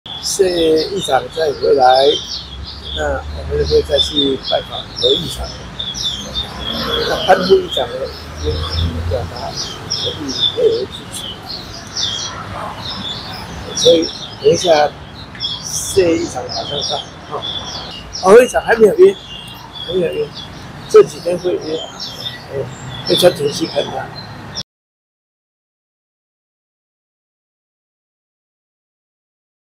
與謝典林會面 王金平：挺郭派差不多都歸隊
彰化縣議長謝典林今日上午到台北拜會前立法院長王金平，媒體詢問是否希望謝典林回歸藍軍，王金平表示，這是主要目的；至於是否有信心挺郭派都會歸隊？王金平說：「應該都差不多了吧」，除了謝典林，會再去拜訪南投縣議長何勝豐，何還沒有約時間，這幾天會約，專程去看他。